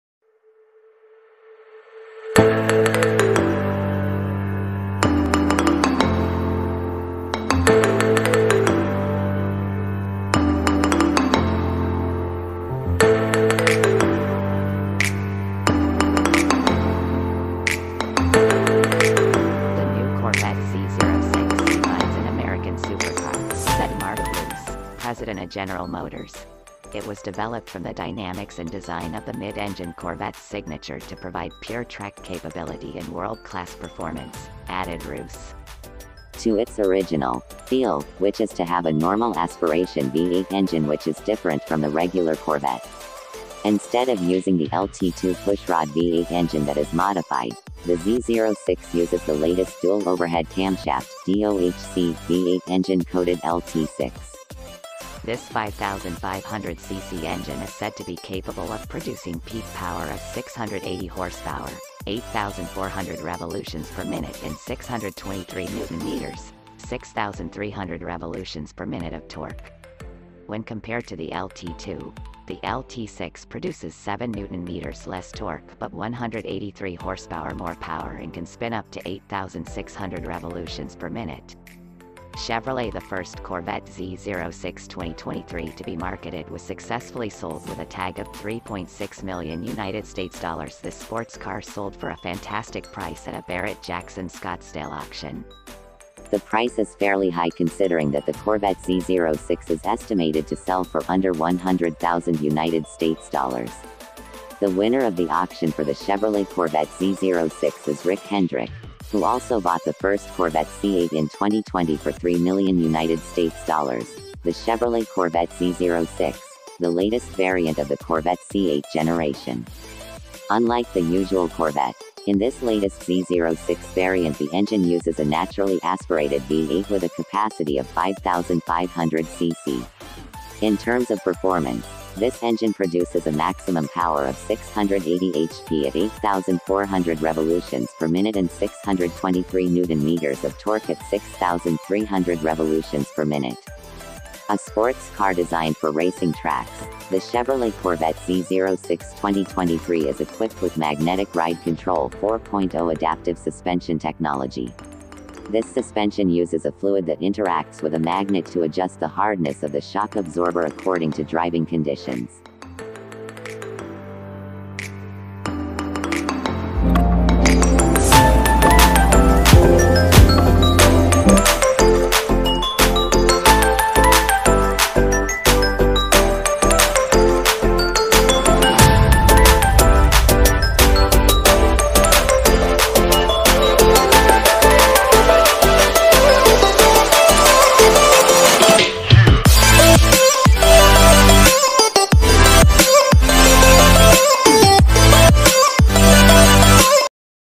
2023 New Chevrolet Corvette Z06
This 5,500 cc engine is said to be capable of producing peak power of 680 hp / 8,400 rpm and torque of 623 Nm / 6,300 rpm.